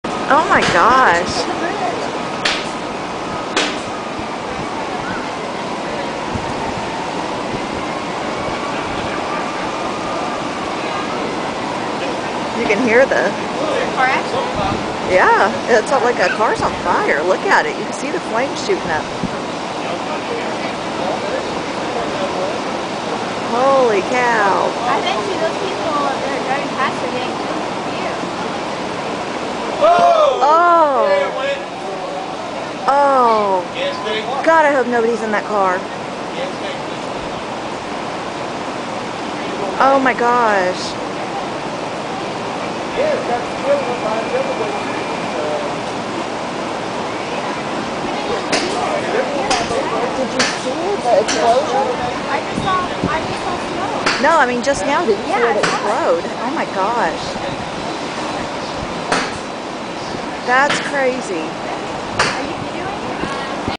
Car accident in the Occoquan
having lunch today and captured a car accident that resulted in a firey explosion.